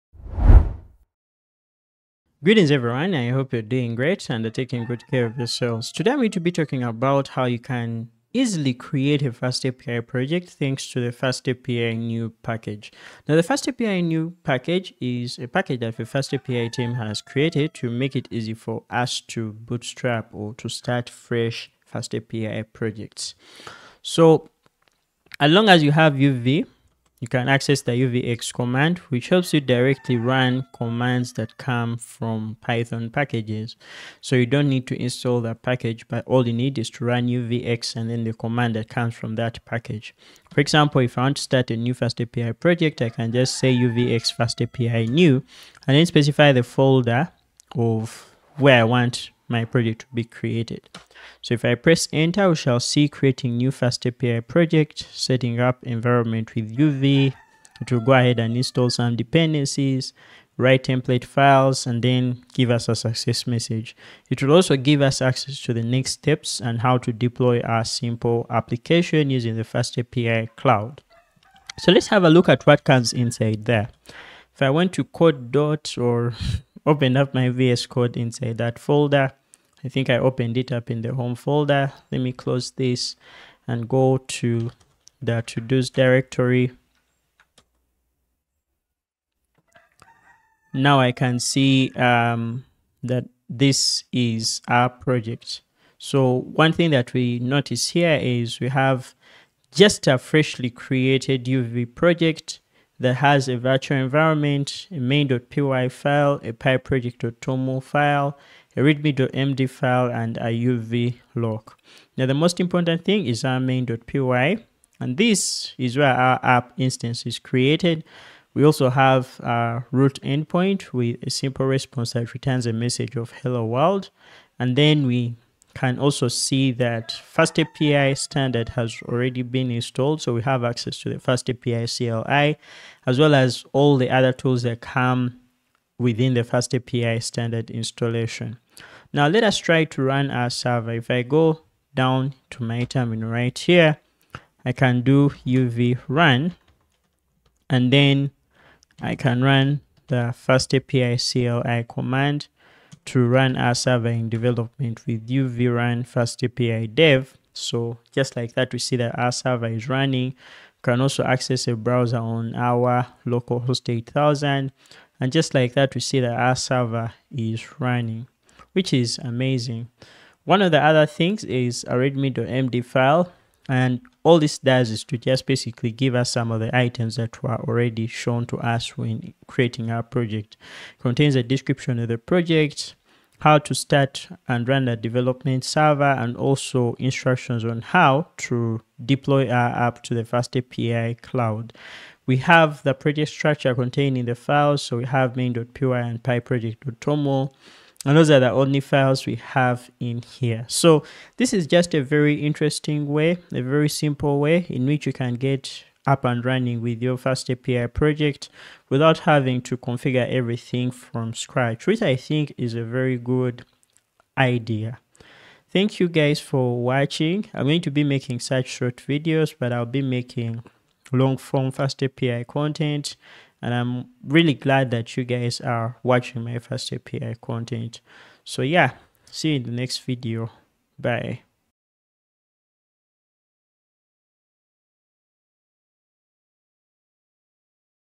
Easily Set Up a Fresh FastAPI Project with the fast new command
Through the fastapi-new package, you can easily create a new FastAPI project with UV. This makes project setup smooth allowing you to start your projects without hustle

You can also consider Becoming a member of this channel.